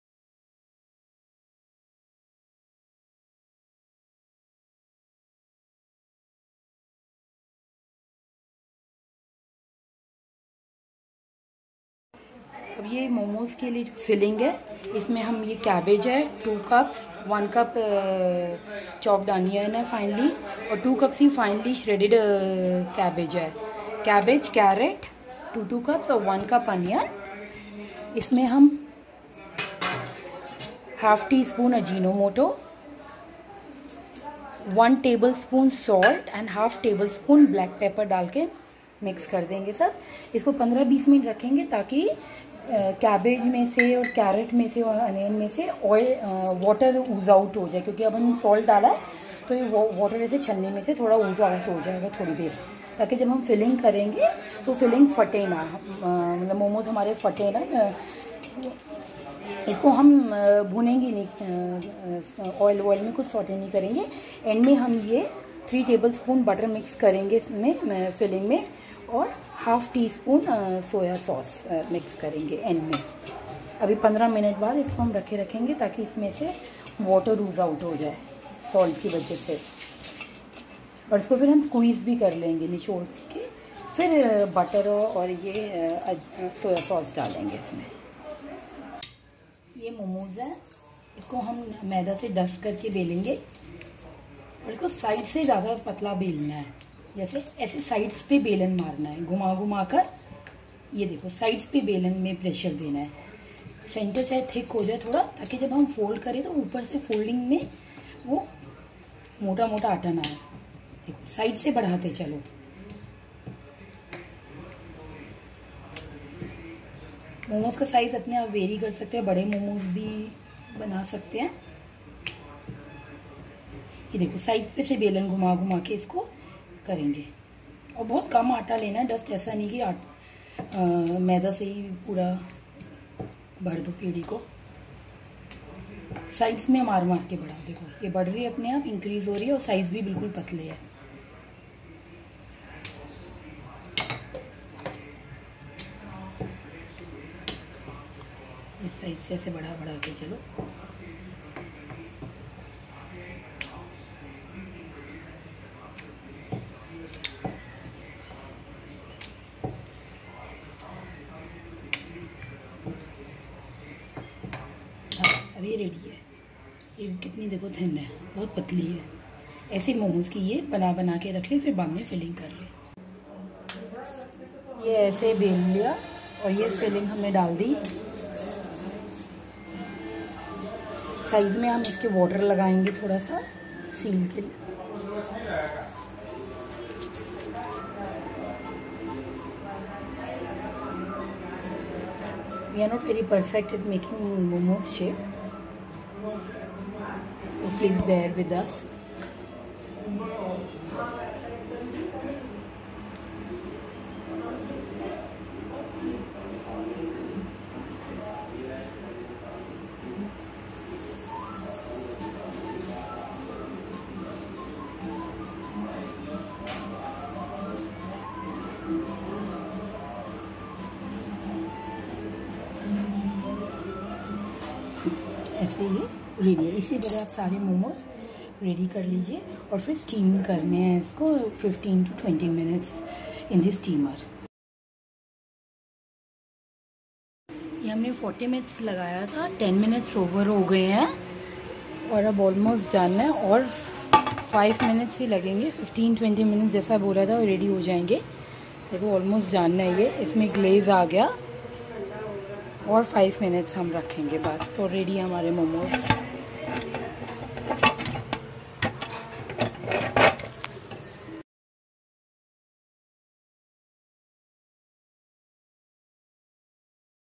Delicious Homemade Momos
Ingredients:

For dough

Maida 3cups
Salt 1tbsp
Oil 2tbsp
Baking pw 1tsp
Water 1cup+5tbsp

For filling:

Shredded cabbage- 2cups
Finely chopped carrot -2cups
Finely chopped Onion -1cup
Ajinomotto 1/2 tsp
Soya sauce 1tsp
Melted butter 3bsp
Salt 1.5tbsp
Black pepper pw 1/2 tbsp

Thank u😊